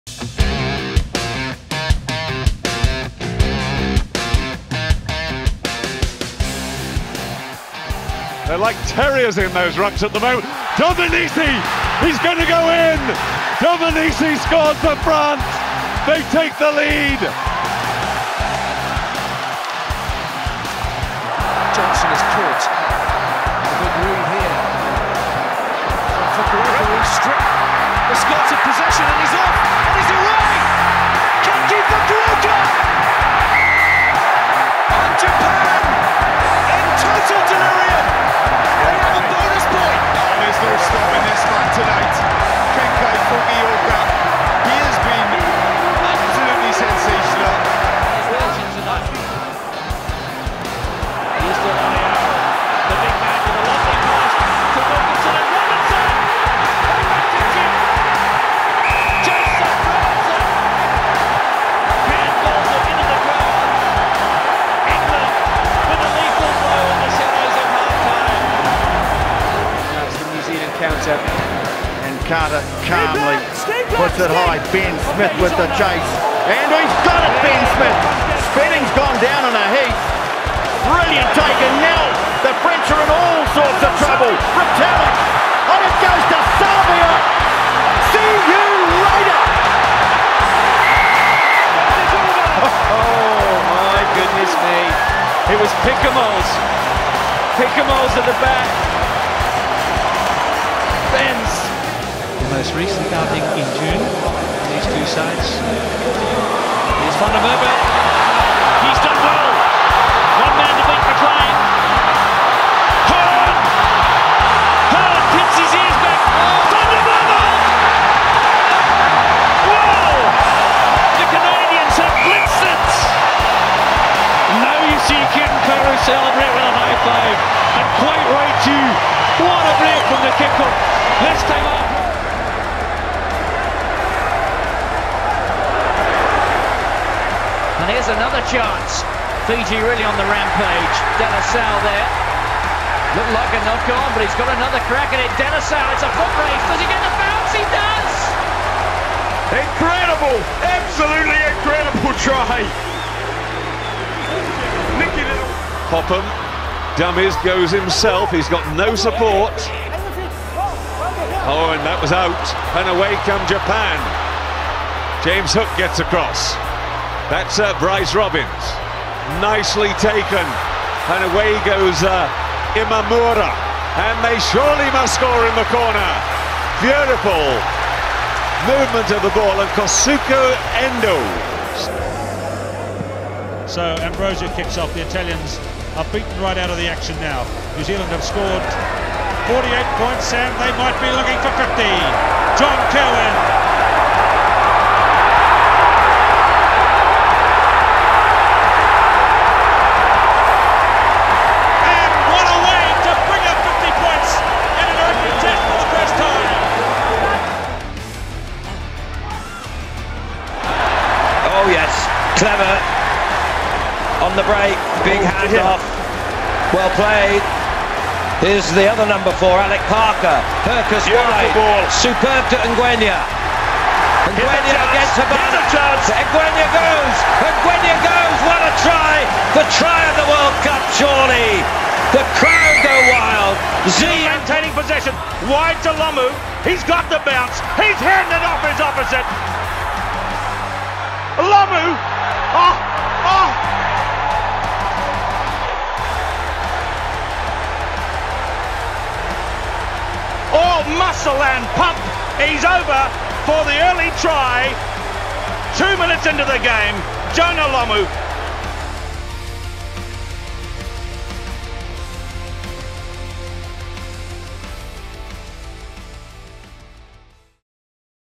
Top 10 Wing Tries in Rugby World Cup History | Lomu, Savea, Robinson & More
Which one was your favourite? Let us know below!

We look at the top 10 tries scored by wingers in Rugby World Cup history, including top efforts from New Zealand's Jonah Lomu, Julian Savea and England's Jason Robinson.